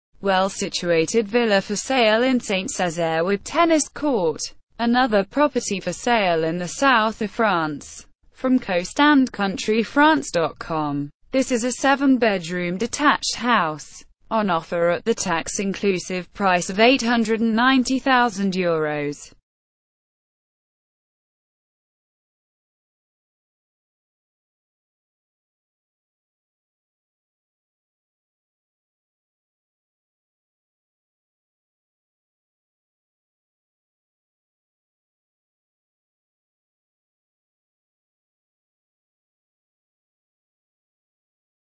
Well situated villa for sale in Saint Cezaire with tennis court
A 7 bedroom Detached house for sale at 890,000 euros. Ref: 3345MO-GH